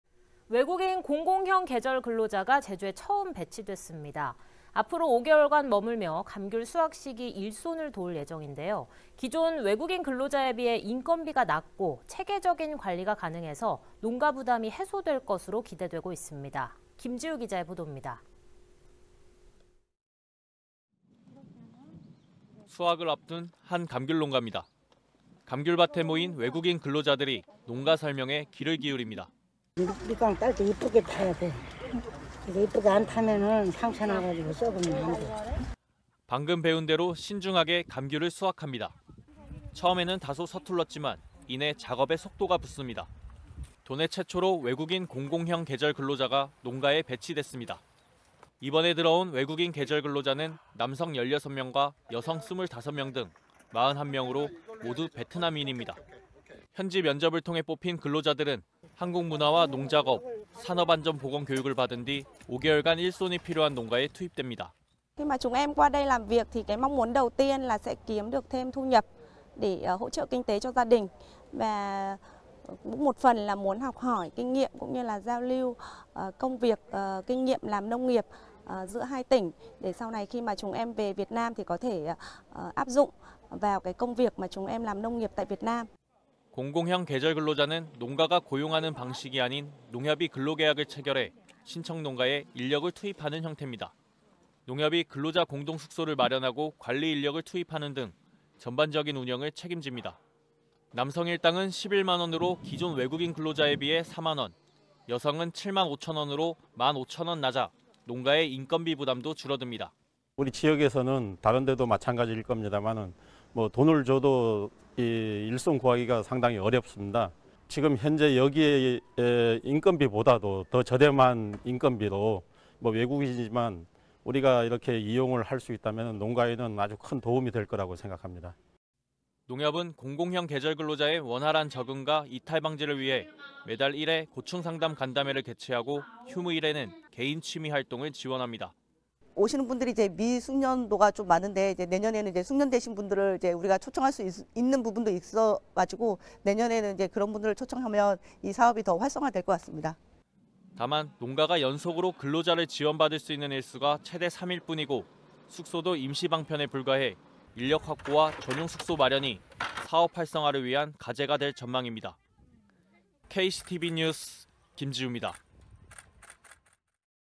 "귤 따러 왔어요" 공공형 계절근로자 첫 배치
외국인 공공형 계절근로자가 제주에 처음 배치됐습니다.

5개월간 머물며 감귤 수확시기 일손을 도울 예정인데요.

기존 외국인 근로자에 비해 인건비가 낮고 체계적인 관리가 가능해 농가 부담이 해소될 것으로 기대되고 있습니다.